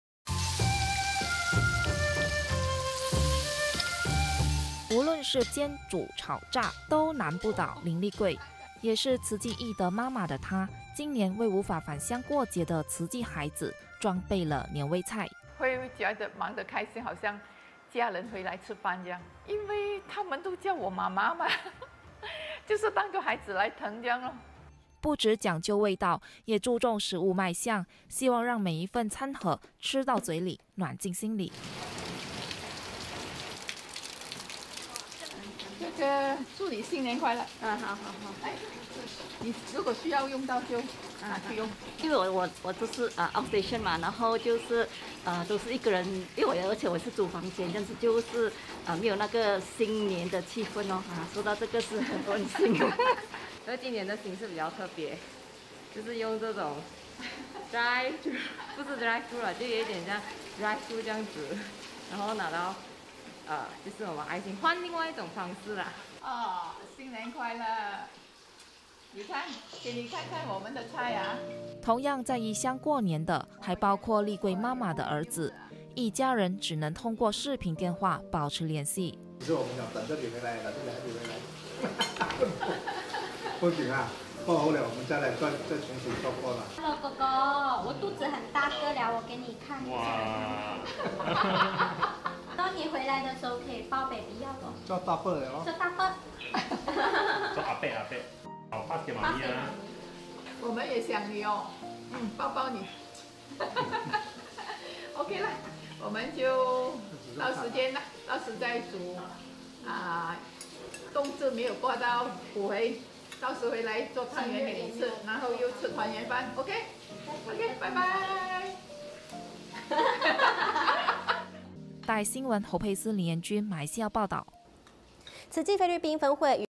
大馬MCO禁跨州 懿德孩子另類圍爐
面對疫情，不能回家團員怎麼辦，於是他準備了另類圍爐，無論是煎、煮、炒、炸，都難不倒林麗桂，也是慈濟懿德媽媽的她今年為無法返鄉過節的慈濟孩子裝備了年味菜。

慈濟志工 林麗桂：「會覺得忙得很開心，好像家人回來吃飯這樣，因為他們都叫我媽媽，就是當做(自己)孩子來疼這樣。」

不只講究味道，也注重食物賣相，希望讓每一份餐盒吃到嘴裡暖進心裡。

志工 李采蓉：「因為我都是外地人，然後就是都是一個人，而且我又是租房間，就是沒有那個新年的氣氛，收到這個是很溫馨的。」

慈青學長 吳佩恩：「今年的形式比較特別，就是用這種得來速，不是得來速，就有點像得來速，然後拿到媽媽的愛心，換另外一種方式。」

同樣在異鄉過年的，還包括麗桂媽媽的兒子，一家人只能通過視頻電話保持聯繫。

林麗桂的先生：「 其實我們想等著你回來，可是你還沒有回來，不用緊啦 過後了，我們再重新做過。」

林麗桂的媳婦：「哈囉哥哥 我肚子很大個了，我給你看，所以你回來的時候，可以抱寶寶了 做大伯了，他想媽咪了。」

慈濟志工 林麗桂：「我們也很想你哦，抱抱你，Ok啦 我們就到時見，到時再煮，冬至沒有過到補回，到時回來做湯圓給你吃，然後又吃團圓飯ok，掰掰。」

侯佩思 林妍君 馬來西亞報導